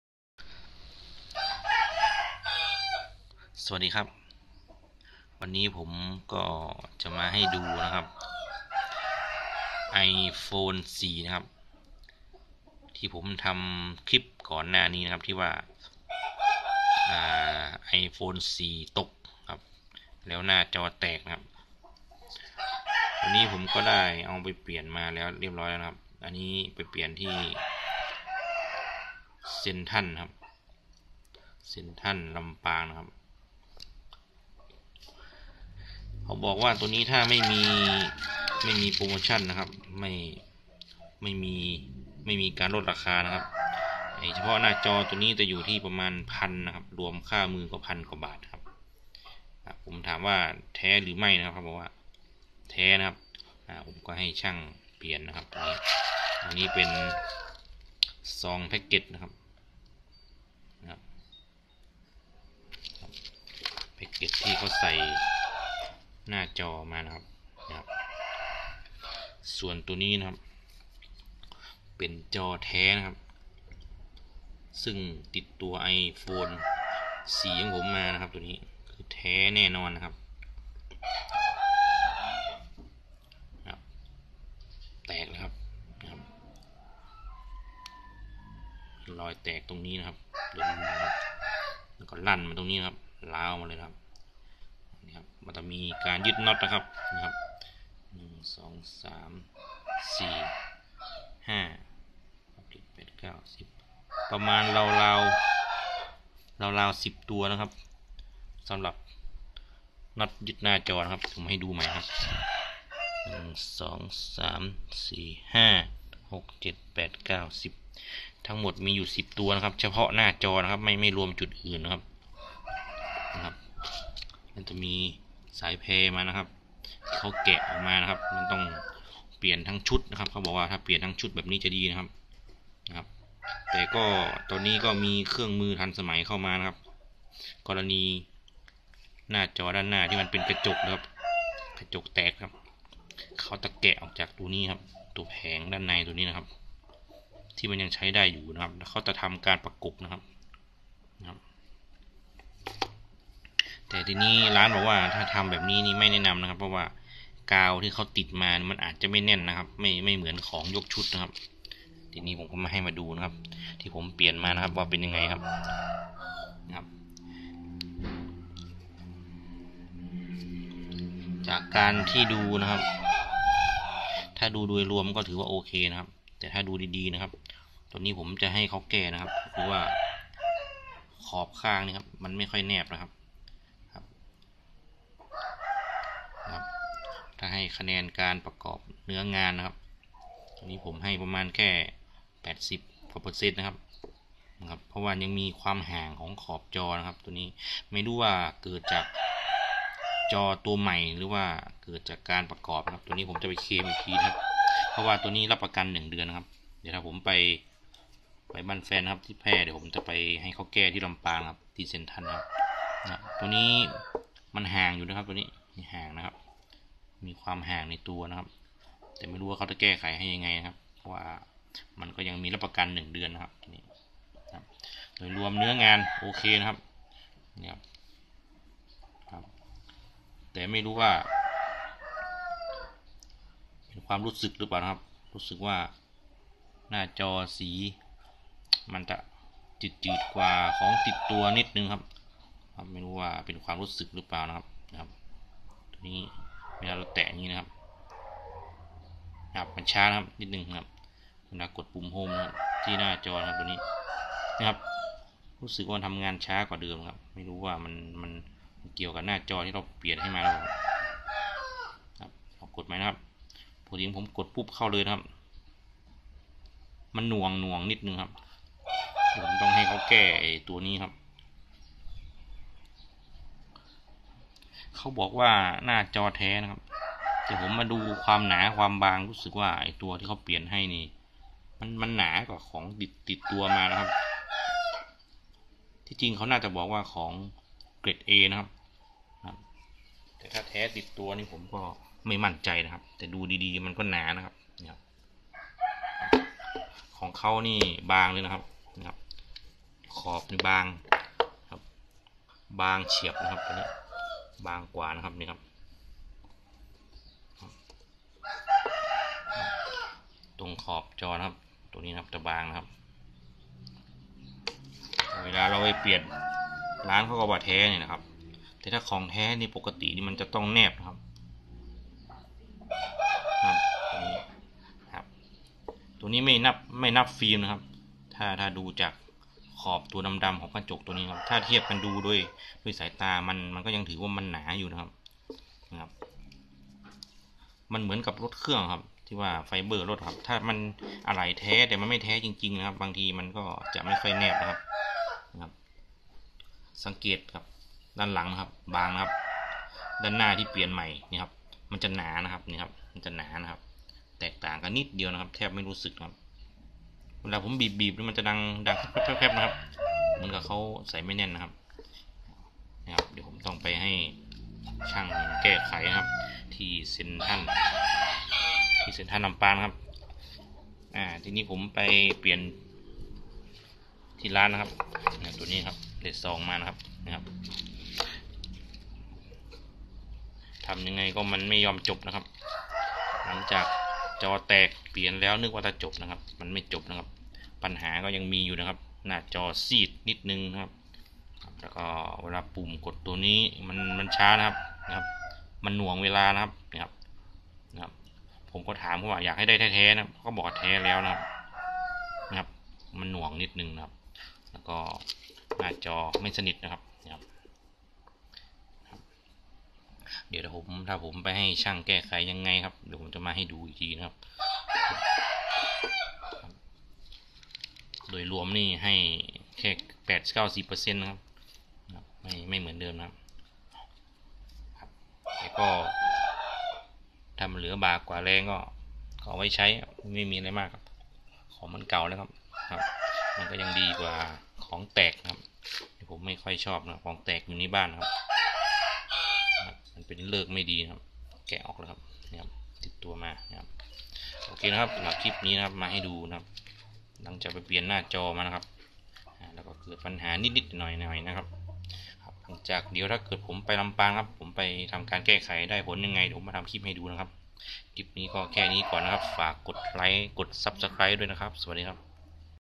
ไอโฟน4หน้าจอแตก
ไอโฟน4หลังเปลี่ยนหน้าจอเป็นยังไงมาดูกันงานนี้ยังไม่จบจอยังไม่ค่อยแนบเนียนเท่าไหร่